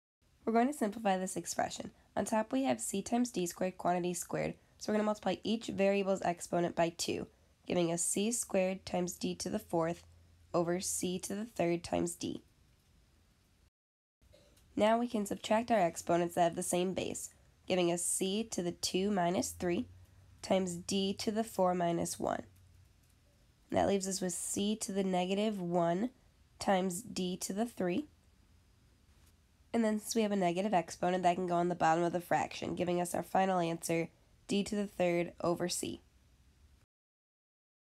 Exponents: Simplify (c d^2)^2 / c^3 d
Megan from SVSU Micro Math helps you simplify an expression involving exponents using properties of exponents.

Problem: Simplify (c d^2)^2 / c^3 d